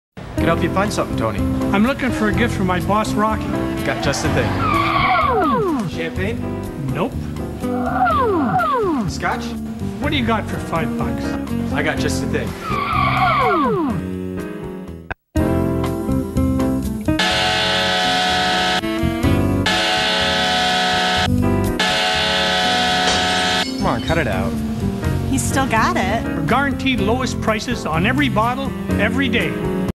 Binny's with Tony Esposito (2010)
Chicago Blackhawks great Tony Esposito shops at Binny's. These are two quite interesting commercials.